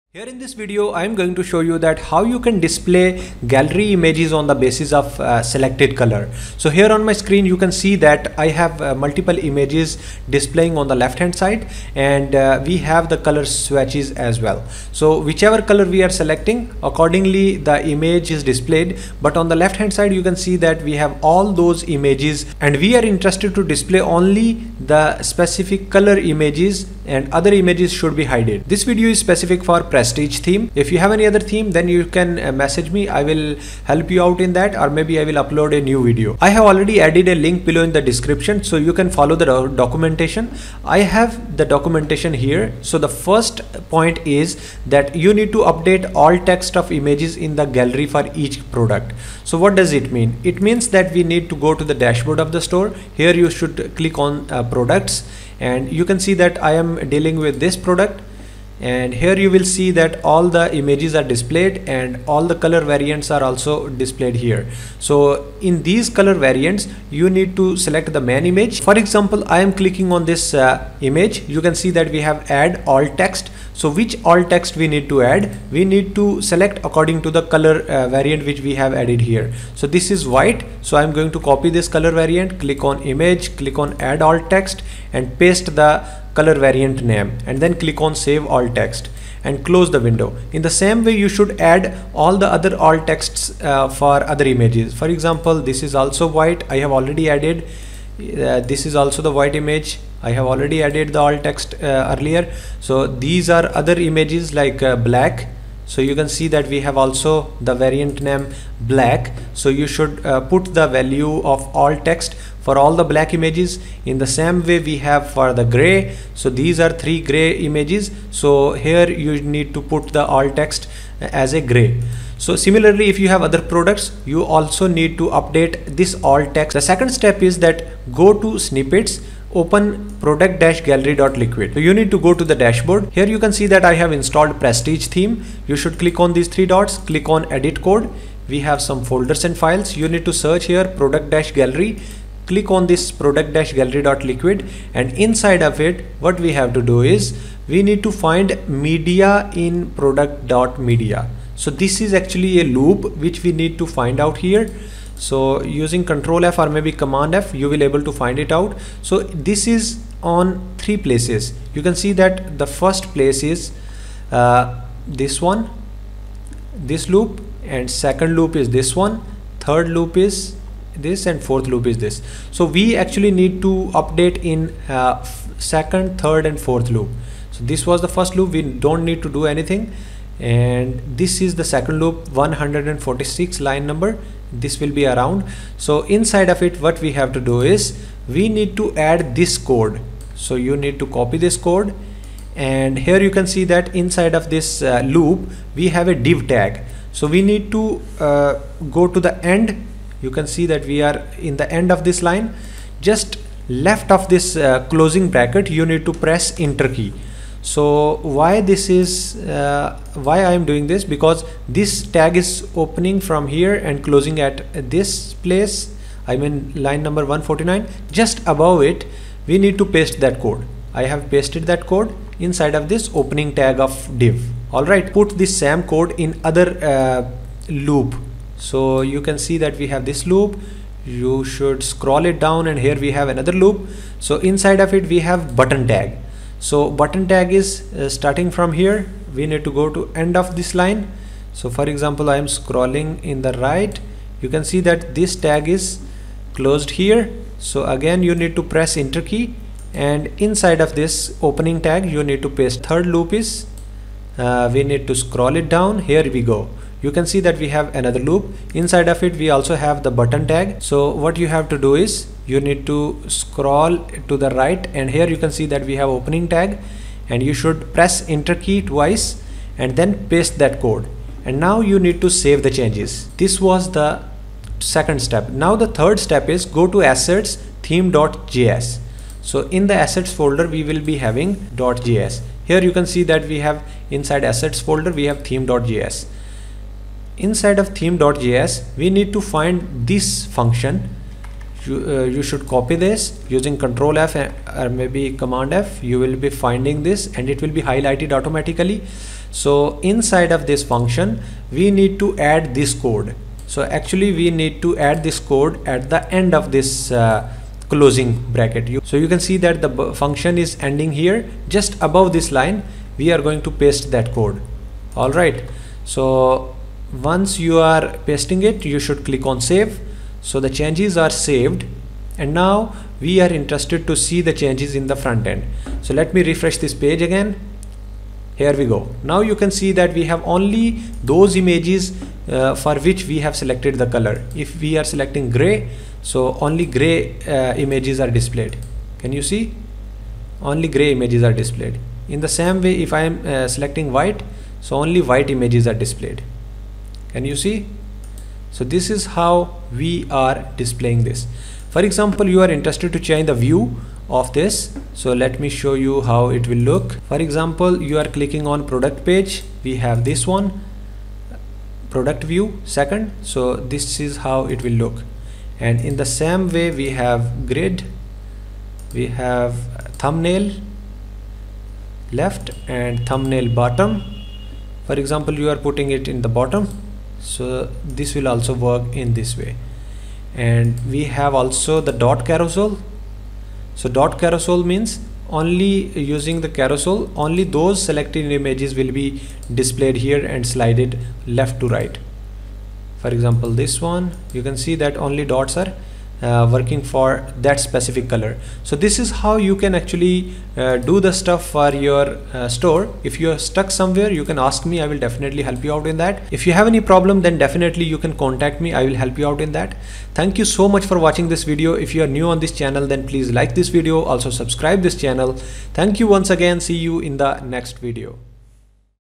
Shopify Display Selected Color Images in Product Page PRESTIGE Theme | Without App [Latest 2024]
Display product images specific to selected variant of the product. You can hide the other color images in shopify.

👋Skype: mohdramzaan112